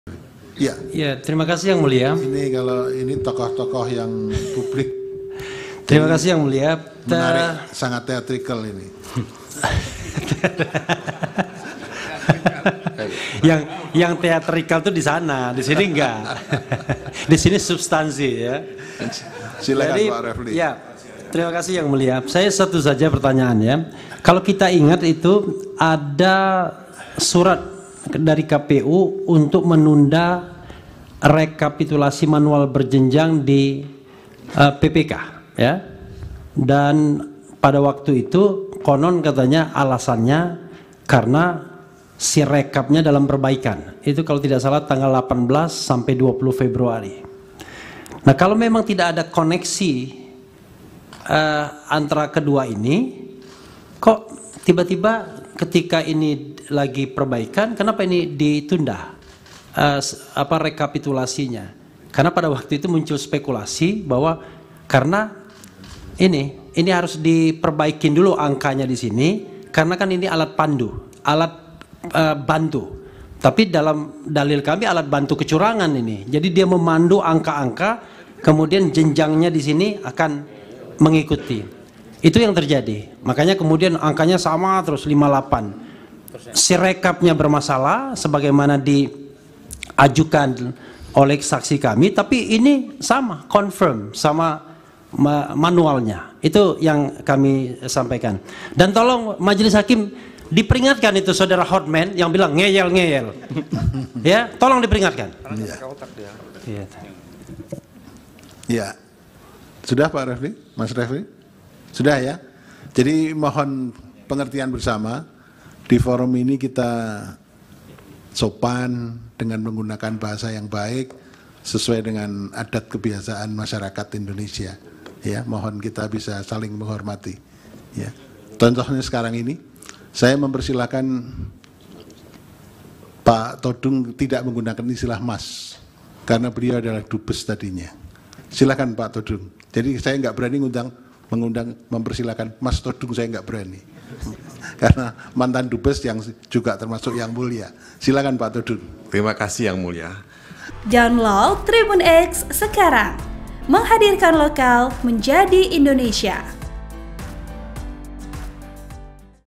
Lawan Hotman Paris di Persidangan, Refly Harun Ngadu ke MK Seusai Tidak Terima Disebut 'Ngeyel'
TRIBUN-VIDEO.COM - Mahkamah Konstitusi (MK) menggelar sidang lanjutan sengketa hasil Pilpres 2024 pada Rabu (3/4/2024) di Gedung MK, Jakarta.

Tak sedikit momen cukup panas mewarnai sidang pada hari ini.

Salah satunya saat kubu 01, Refly Harun 'mengadu' ke Hakim lantaran tak terima disebut ngeyel oleh kubu 02, Hotman Paris.

Refly pun tampak kesal dengan sebutan ngeyel dari Hotman kepadanya yangi dinilai semena-mena.

(Tribun-Video.com/Sandy Yuanita)